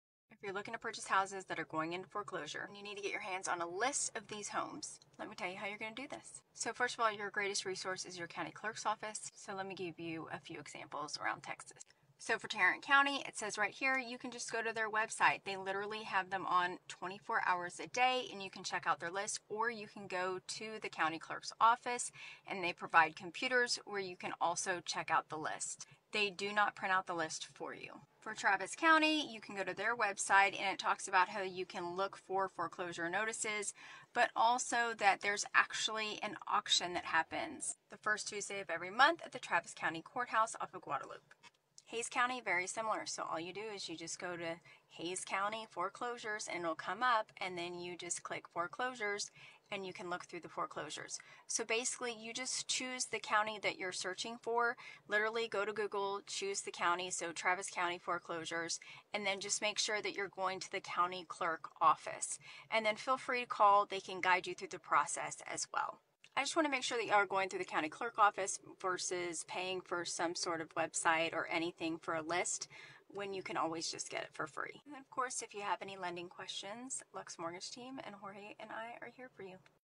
🏡💰 Ready to invest in real estate? Here’s how to get your hands on a list of homes going into forecl
🏡💰 Ready to invest in real estate? Here’s how to get your hands on a list of homes going into foreclosure! 🙌🏼📈